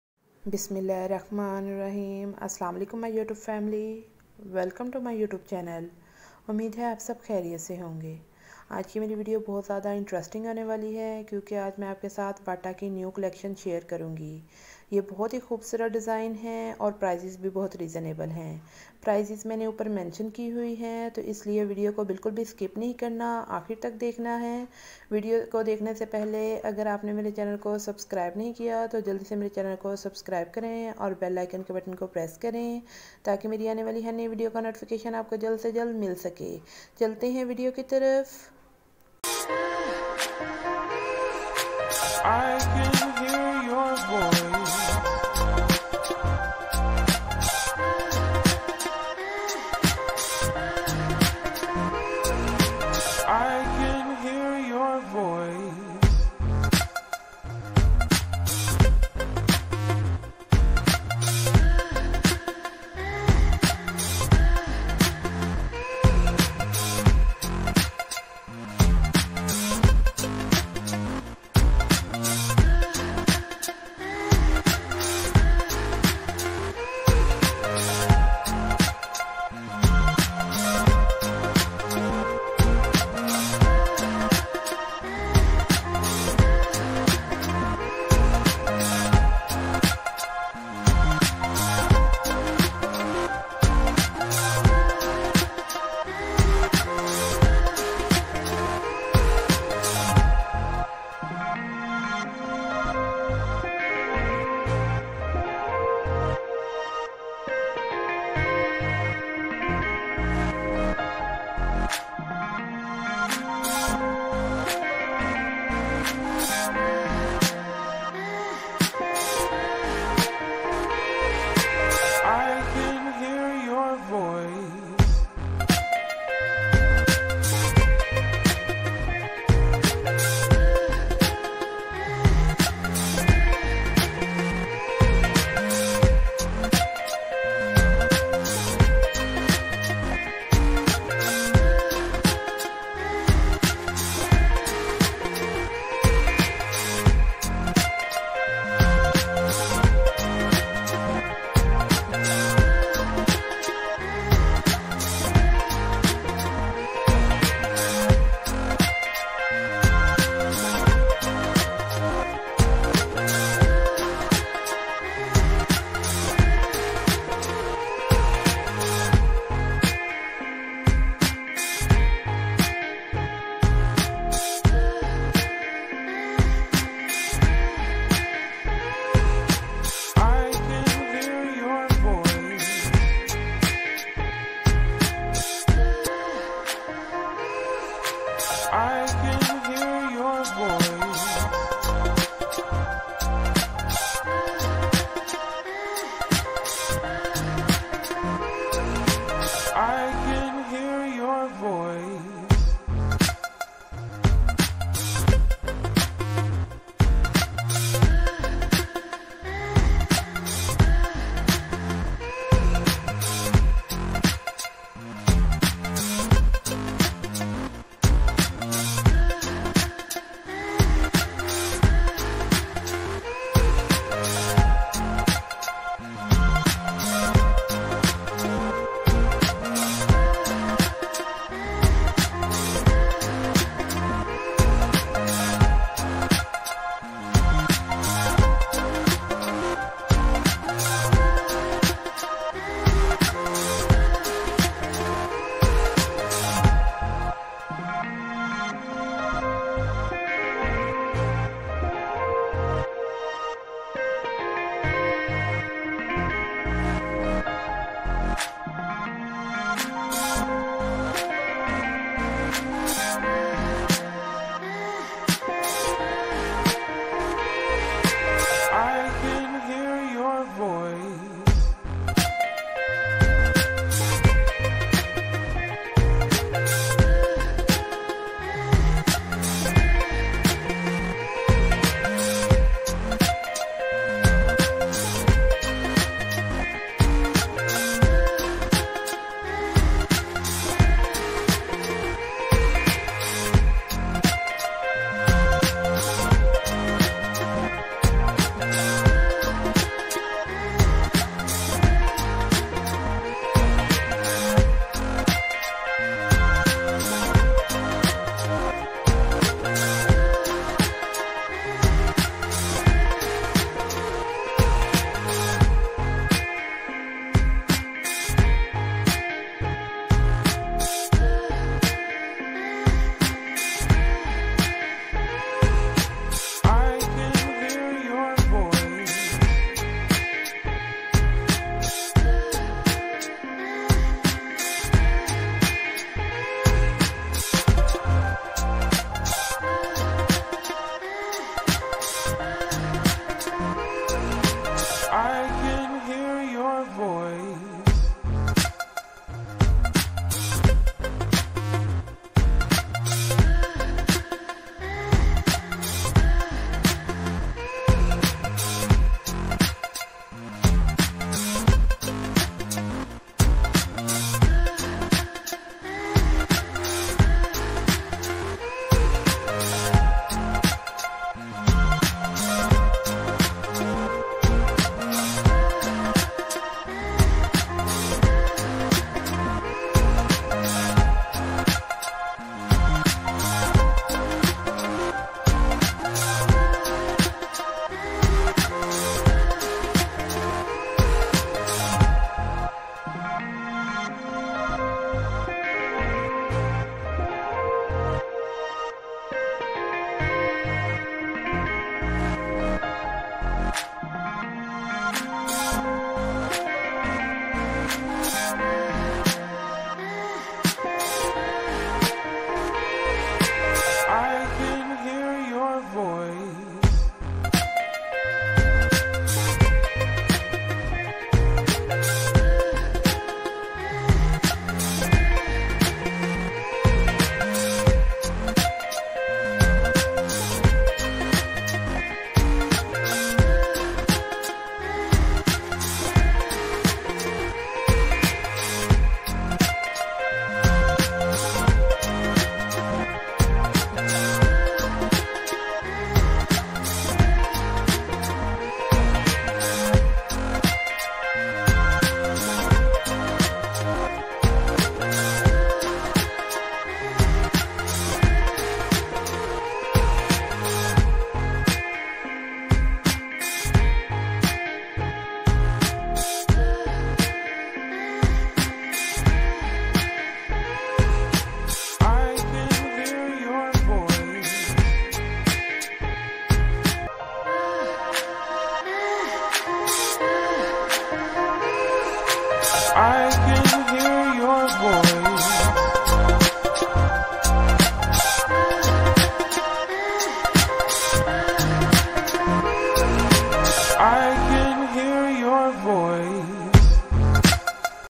Bata Shoes New Collection 22 June 2021 |Bata New Summer Collection |SF Life Diary
Bata Shoes New Collection 22 June 2021 |Bata New Summer Collection
SF Life Diary